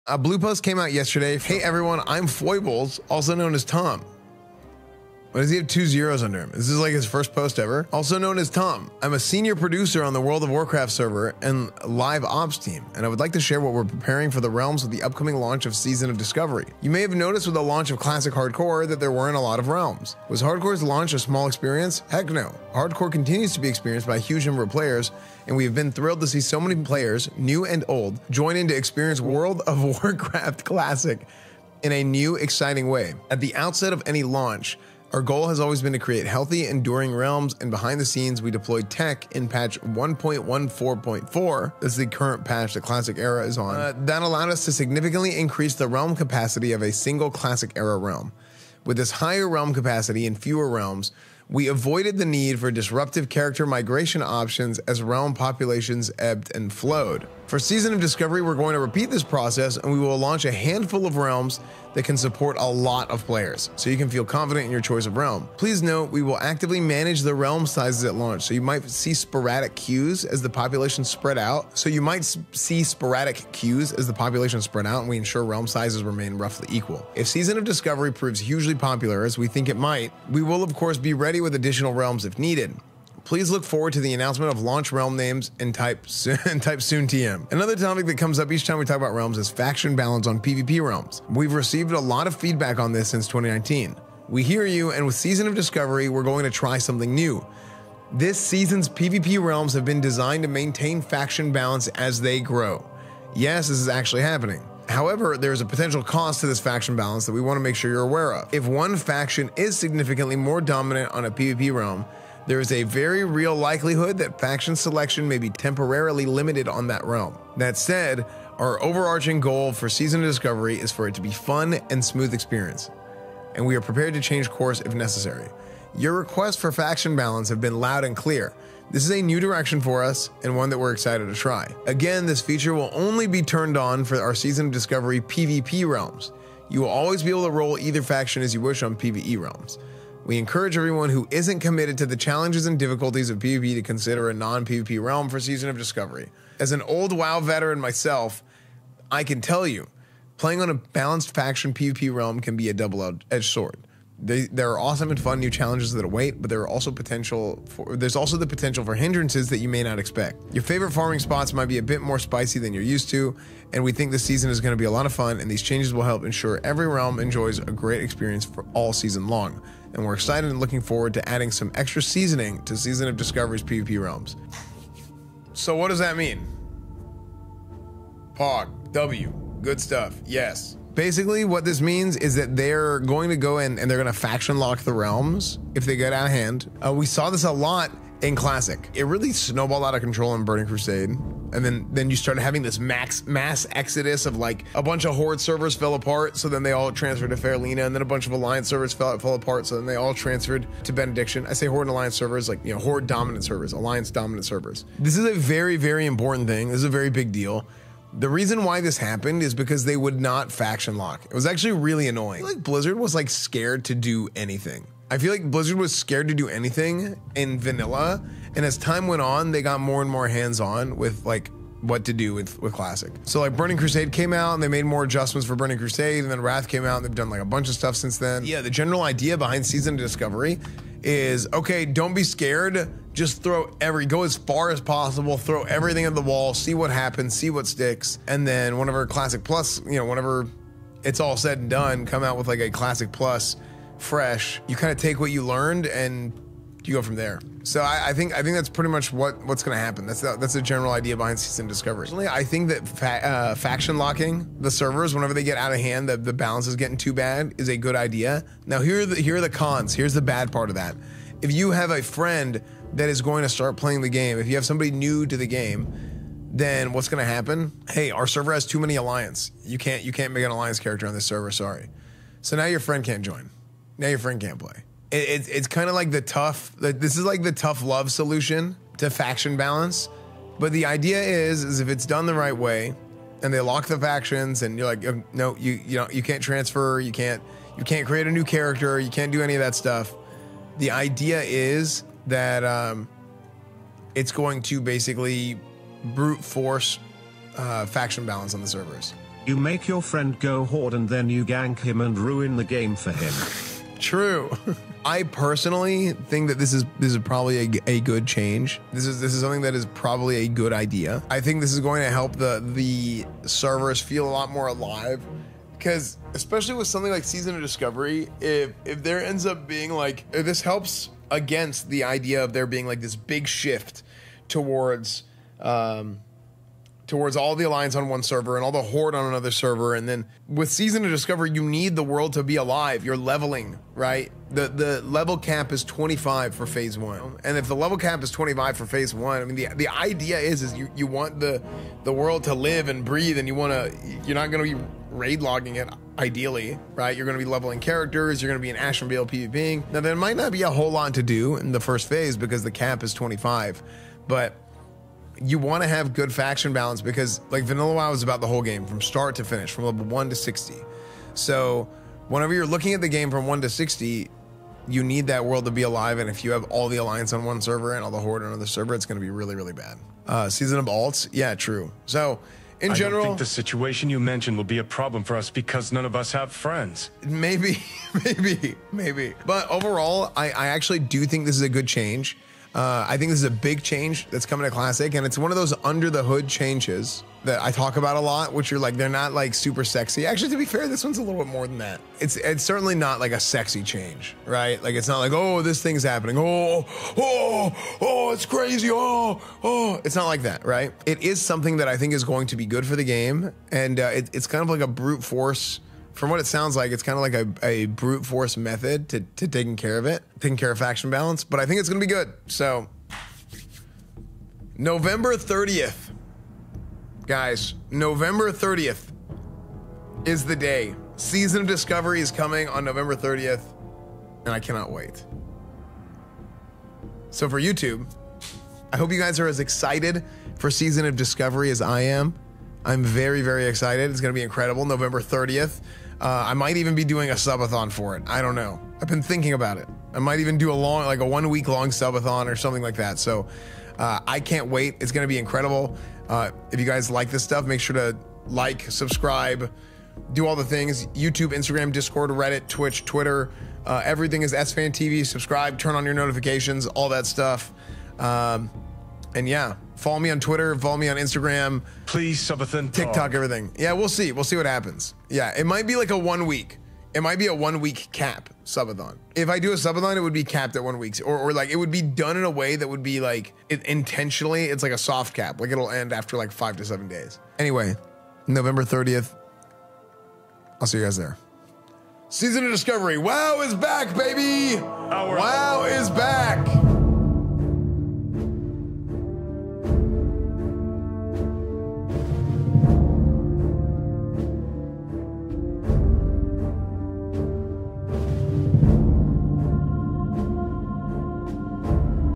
Big Win For Season Of Discovery
Giving my thoughts to Season of Discovery blue post announcement that just came up on Blizzard's plans on SoD servers and their philosophy behind Classic Plus server management & faction balance optimization.

Starforge: The best PCs in the universe.

keychain.club/Esfand